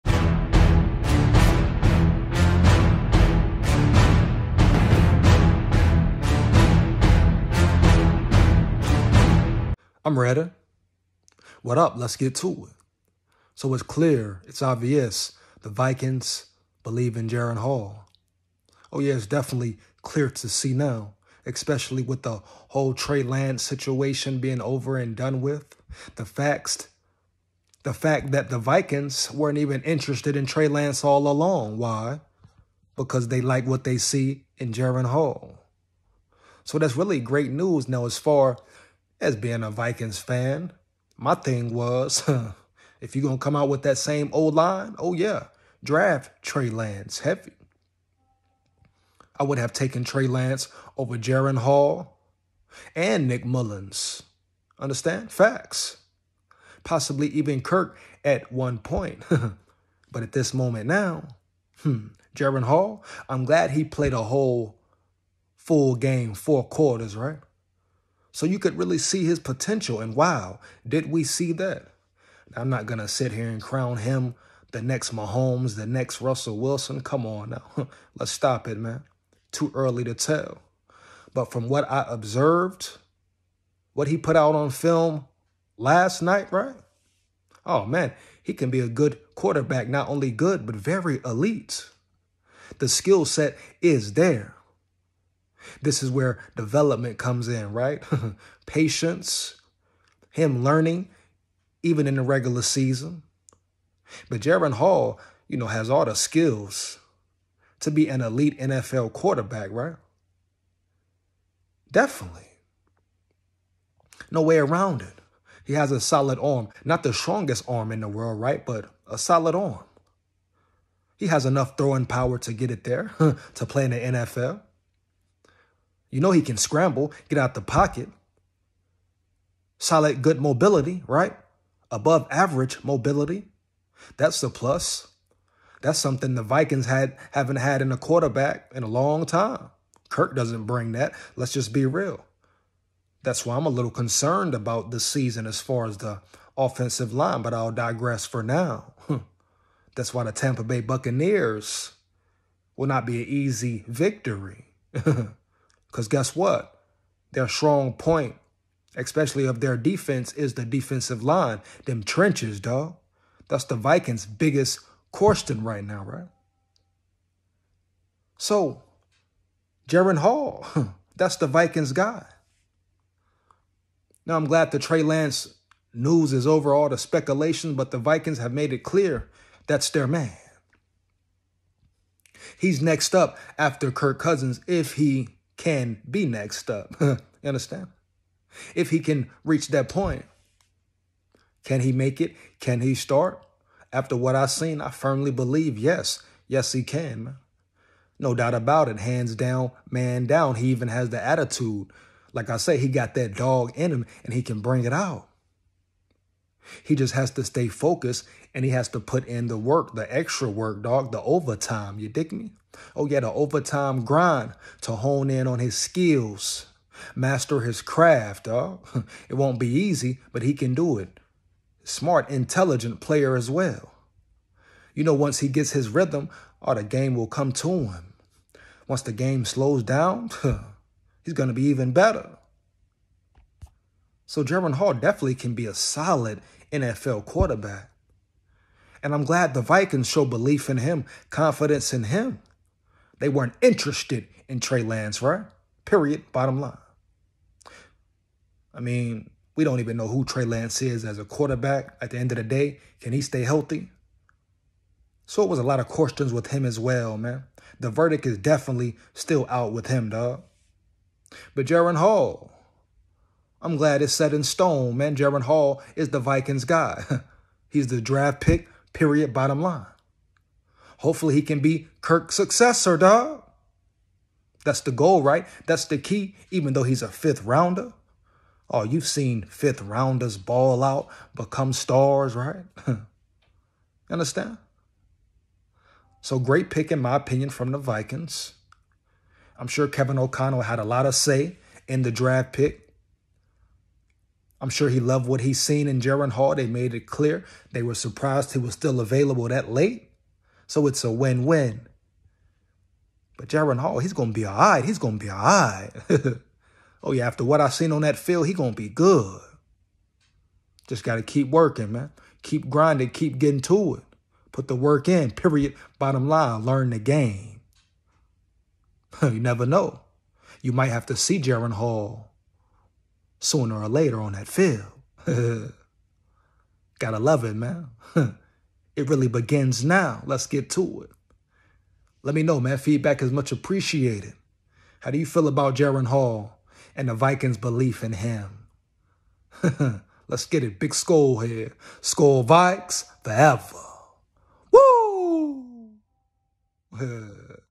The Minnesota Vikings were not THINKING about Trey Lance.They BELIEVE in Jaren Hall.SKOL VIKES..!!
The Minnesota Vikings..
Did not want..
Trey Lance..
They believe in Jaren Hall..
And now..
They have a good reason..
To believe in him..
Hands down man down..
He can be ELITE..
Or he can be average..
Time will tell..
HEAVY..